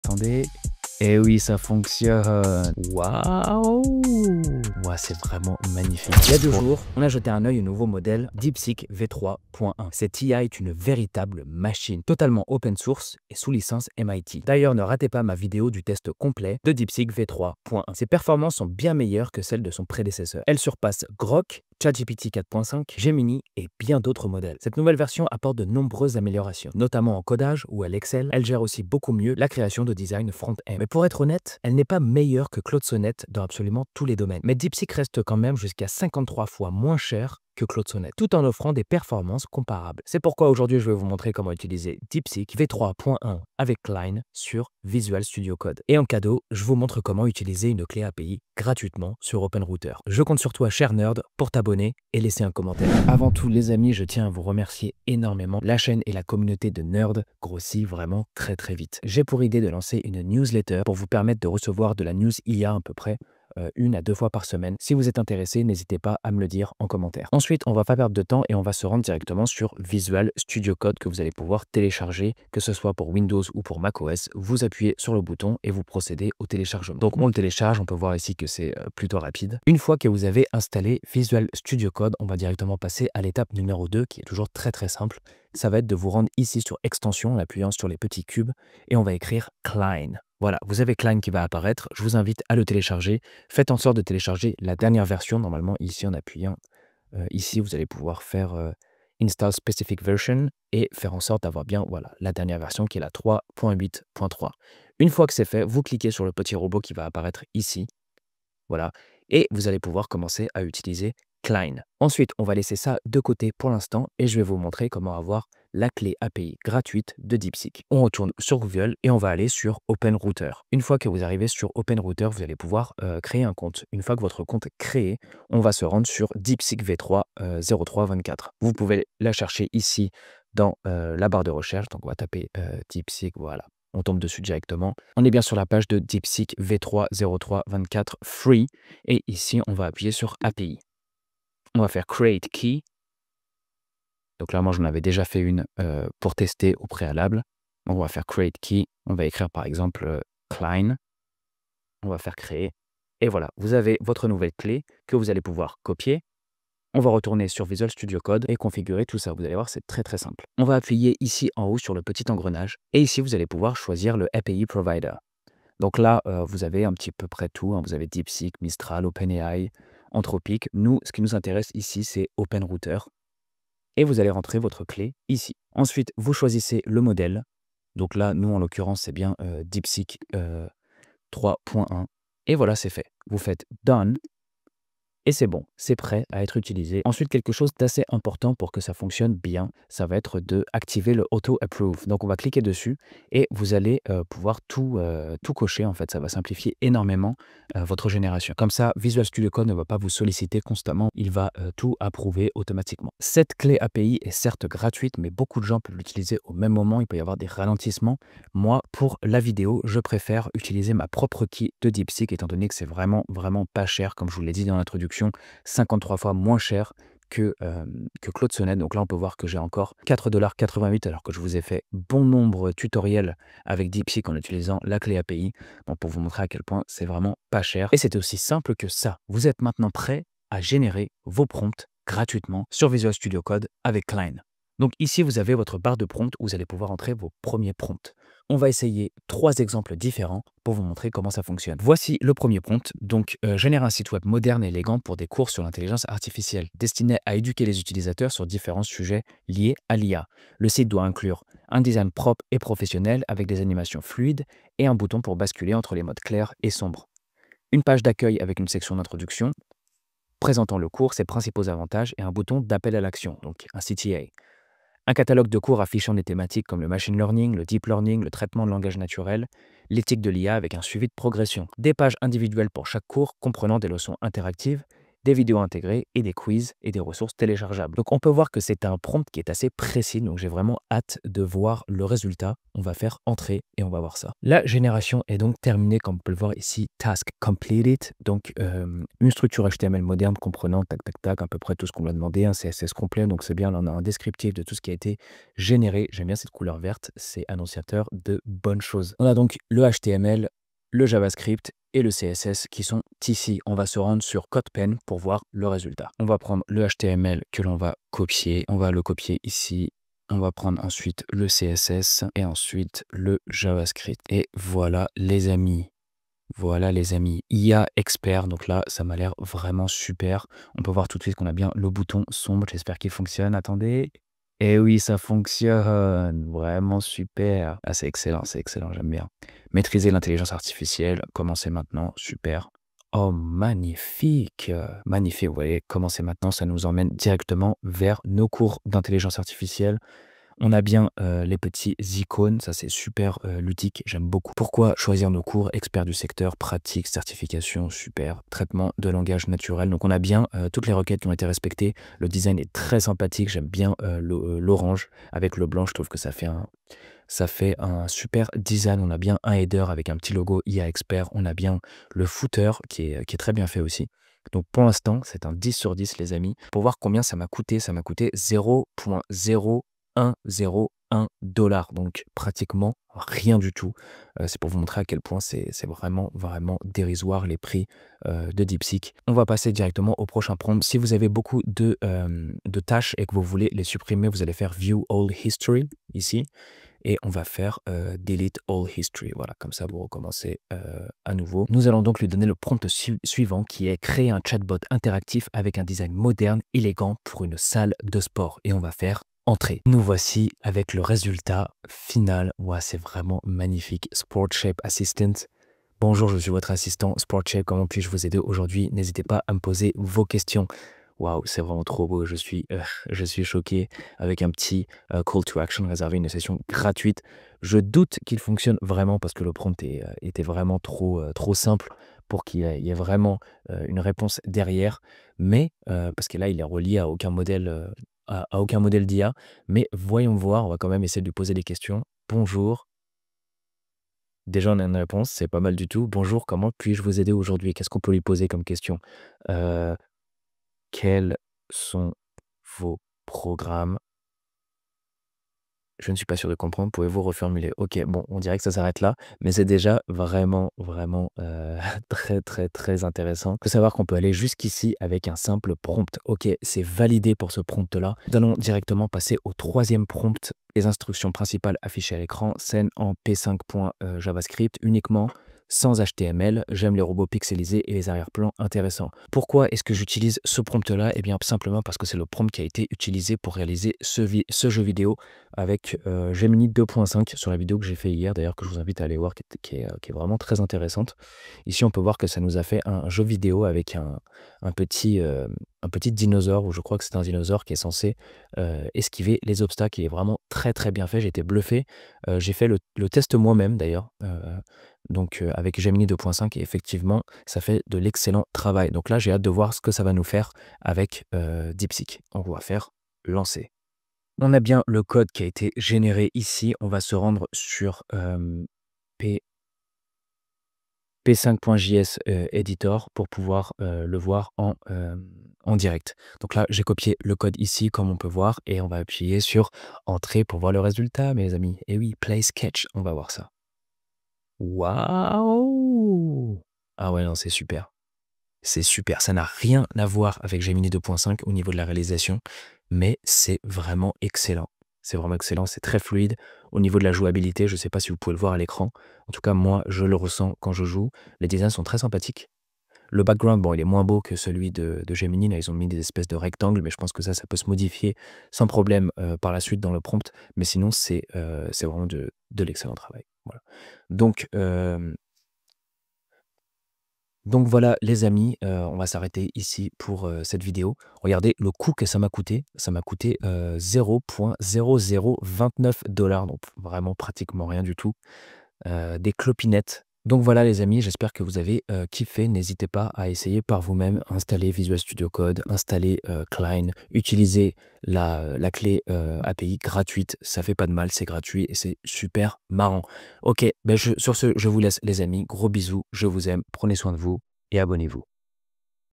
Deepseek V3.1 + Cline : Créez Tout Ce Que Vous Voulez GRATUITEMENT 🤯
🔥 Découvrez comment utiliser Deepseek V3.1 avec Cline pour créer tout ce que vous voulez gratuitement : images, textes, code... Une combinaison puissante à ne pas manquer ! 💥

SOMMAIRE :
00:00 - Introduction
00:52 - Installation VSC + Cline
04:00 - Faire un site web
06:10 - Faire un Chatbot
08:00 - Faire un jeu